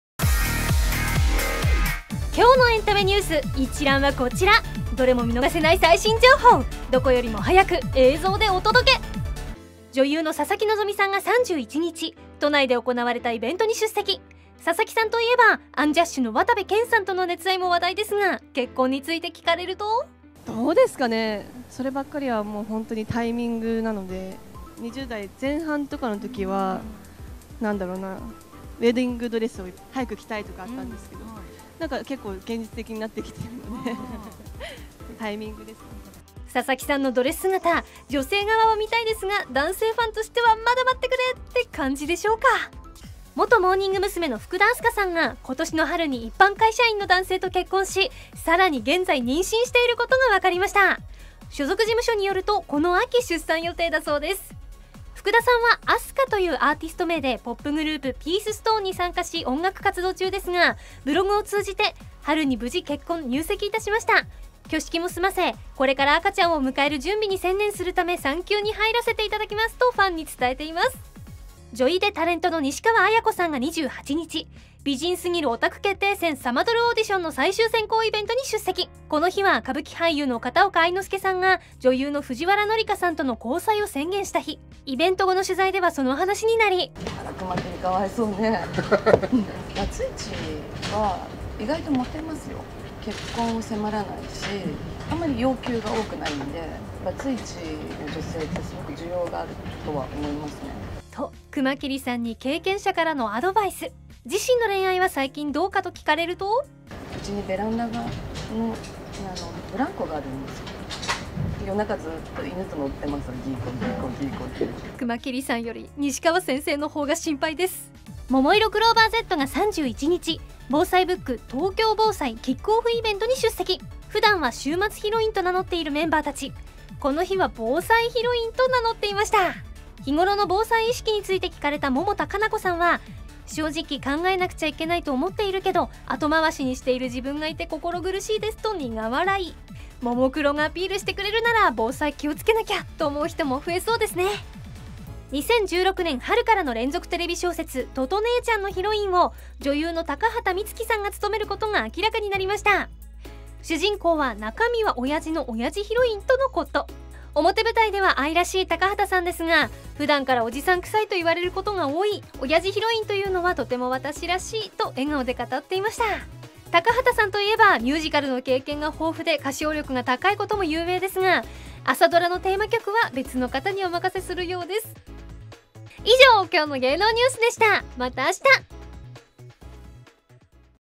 【佐々木希、福田明日香、西川史子、ももいろクローバーᴢ、高畑充希】2015.8.31オリコン芸能ニュース
"ᴏʀɪᴄᴏɴ ᴇɴᴛᴇʀᴛᴀɪɴᴍᴇɴᴛ ɴᴇᴡs" ᴛʜᴀᴛ ᴛᴇʟʟs ᴛʜᴇ ᴛᴏᴘɪᴄᴀʟ ᴇɴᴛᴇʀᴛᴀɪɴᴍᴇɴᴛ ɴᴇᴡs ᴛʜᴀᴛ ᴡᴀs ᴛʜᴇ ᴅᴀʏ ʙʏ ᴘɪᴄᴛᴜʀᴇ. ᴛᴏᴅᴀʏ ɪ ᴡɪʟʟ ᴘɪᴄᴋ ᴜᴘ ɴᴏᴢᴏᴍɪ sᴀsᴀᴋɪ, ᴀsᴜᴋᴀ ғᴜᴋᴜᴅᴀ, ғᴜᴍɪᴋᴏ ɴɪsʜɪᴋᴀᴡᴀ, ᴍᴏᴍᴏɪʀᴏ ᴄʟᴏᴠᴇʀ ᴢ, ᴍᴀᴋɪ ᴛᴀᴋᴀʜᴀᴛᴀ.